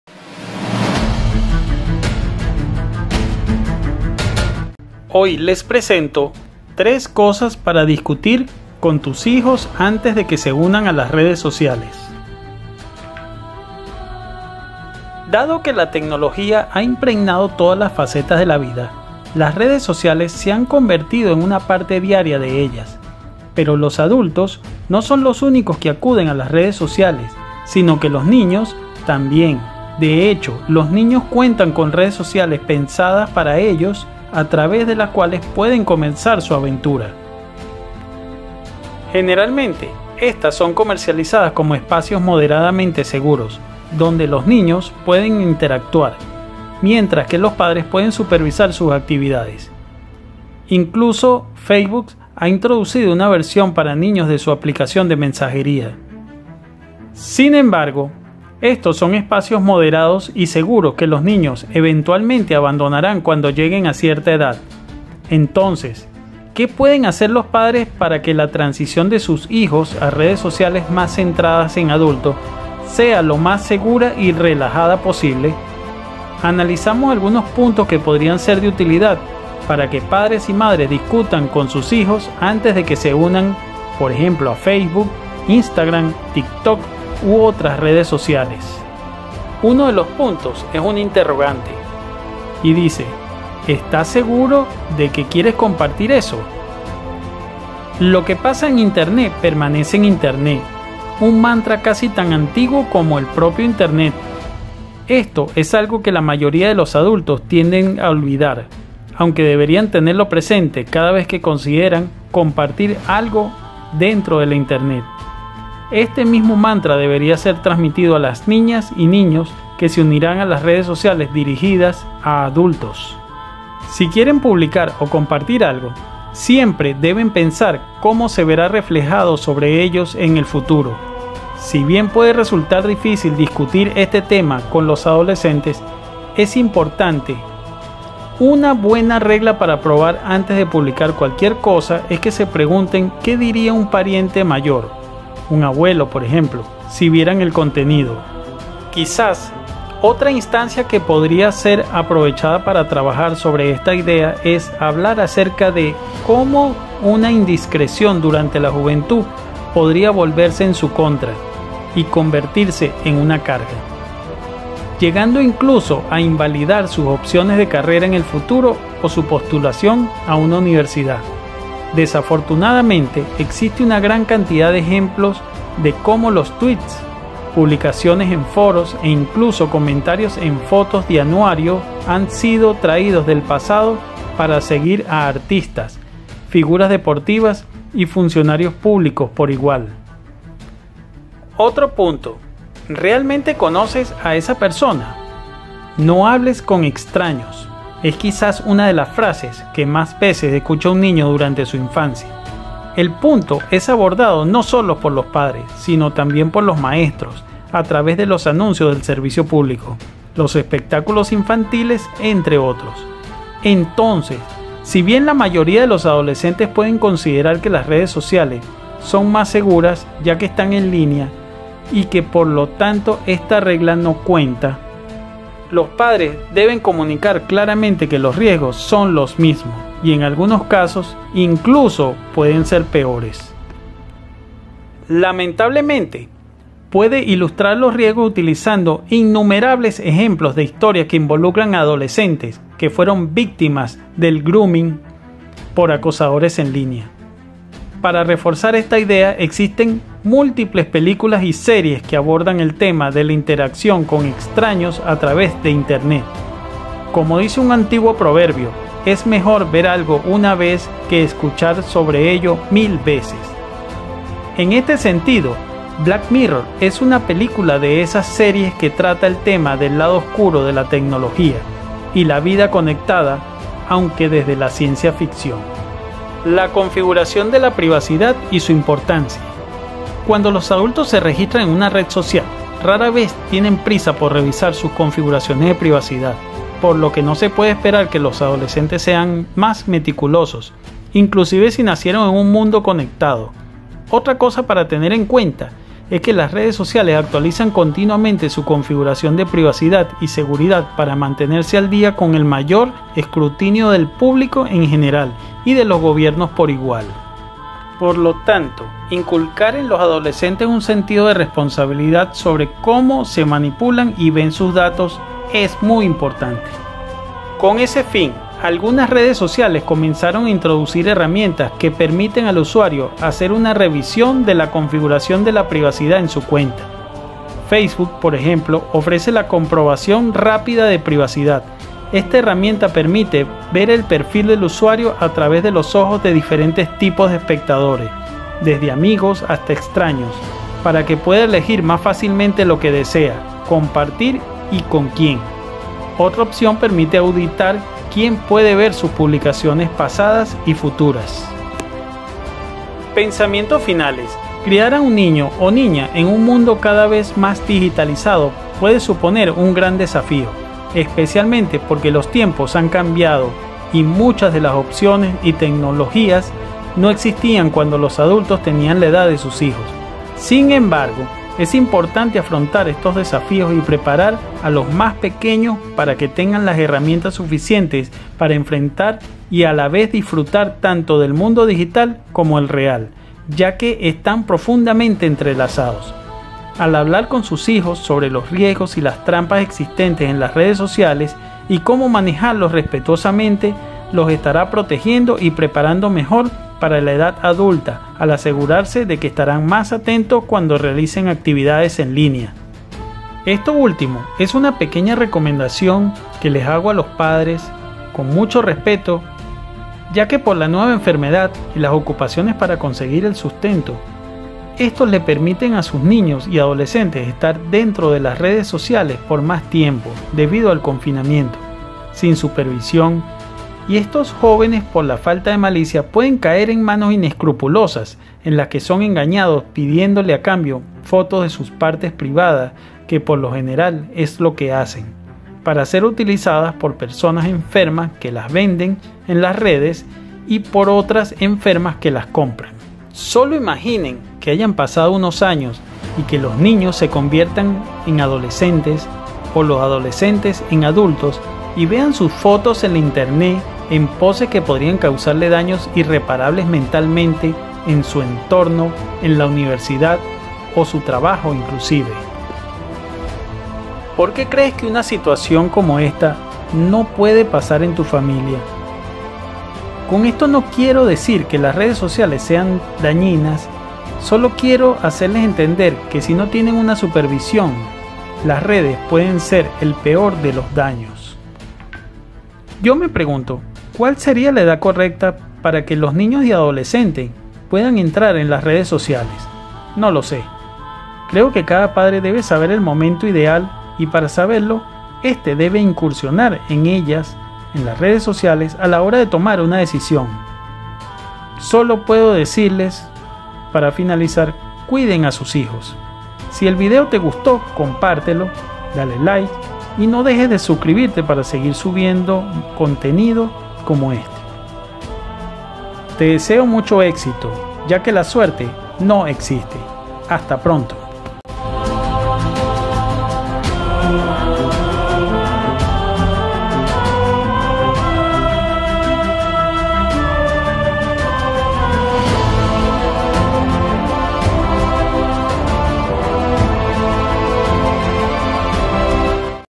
Redes Sociales peligrosas para adolescentes
Quisas para algunos padres la edad para que los niños empiecen a incursionar en las redes sociales sean diferentes, pero lo que si es seguro es que sea la edad que sea deben tener una supervicion inicial para explicarles los peligros que ellos aun no comprenden.

Los peligros en las redes sociales en los niños o adolecentes hacen que sean muy faciles de caer en las trampas de los abusadores, para que estos inexpertos hagan que se tomen fotos sensuales e inclusiven roben a sus padres complaciendo a estos abusadores.